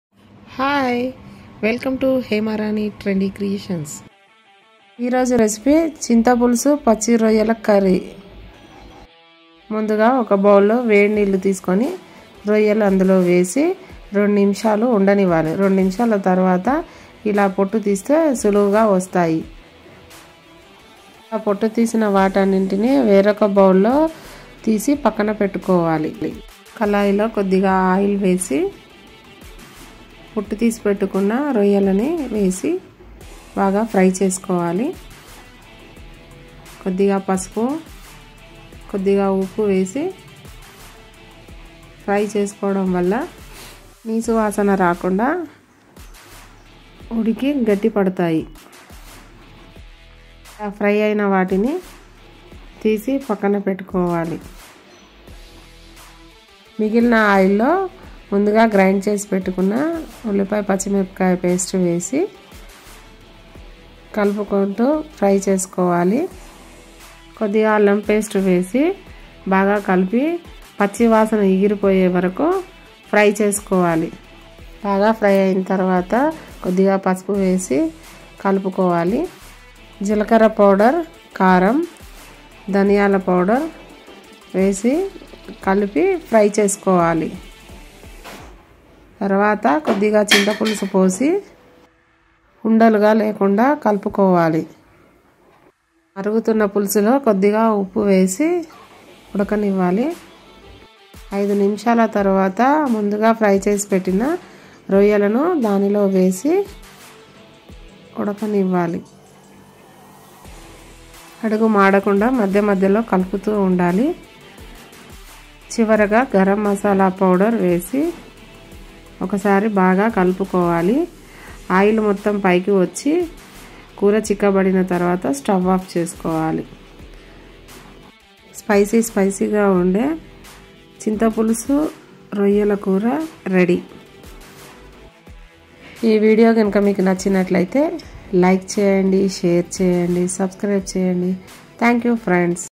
చింతపులుసు పచ్చి రొయ్యల కూర//chintapulusu Pacchi royyala kura
చింతపులుసు పచ్చి రొయ్యల కూర. వేడి నీటిలో వేసి శుభ్రం చేసిన పచ్చి రొయ్యలను కళాయిలో ఆయిల్ వేసి ఫ్రై చేసుకోవాలి. కొద్దిగా పసుపు ఉప్పు వేసుకోవాలి.ఫ్రై అయిన వాటిని తీసి పక్కన పెట్టుకోవాలి. గ్రైండ్ చేసిన ఉల్లిపాయ, పచ్చిమిర్చి పేస్టు వేసి బాగా ఫ్రై చేసుకోవాలి. కొద్దిగా అల్లం పేస్ట వేసి పచ్చివాసన పోయిన తర్వాత జీలకర్ర పొడి, కారం, ధనియాల పొడి వేసి బాగా కలుపుకోవాలి. తర్వాత చింతపులుసు పోసి ఉండలు లేకుండా కలుపుకొని పులుసు ఉడకనివ్వాలి. మరిగినపులుసులో ఫ్రై చేసిన పచ్చి రొయ్యలు వేసి మధ్య మధ్యలో కలుపుతూ ఉండాలి. చివరగా గరంమసాలా వేసి వేసుకోవాలి. ఆయిల్ పైకి తేలిన తరువాత స్టవ్ ఆఫ్ చేసి వేరొక బౌల్ లోకి సర్వ్ చేసుకోవాలి.